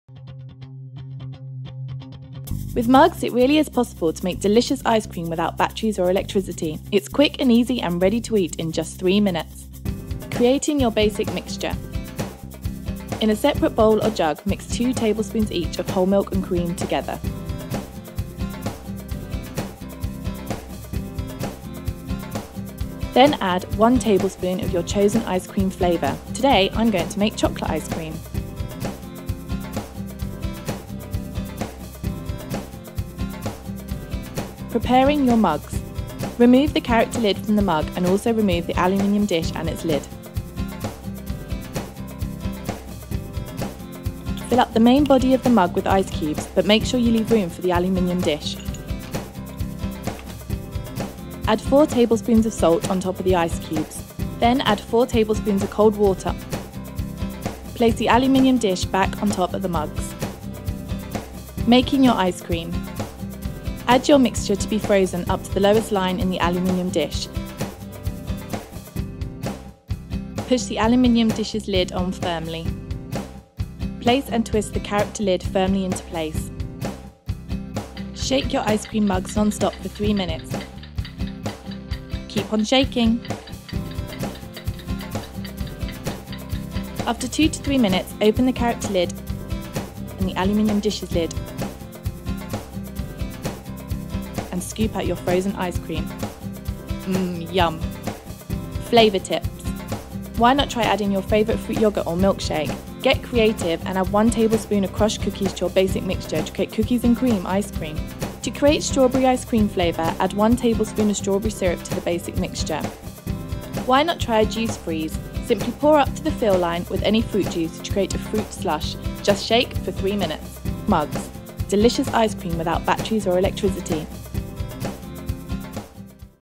Mugz Demo Sizzle
Mugz Demo Sizzle- How to make ice cream in less than 3 minutes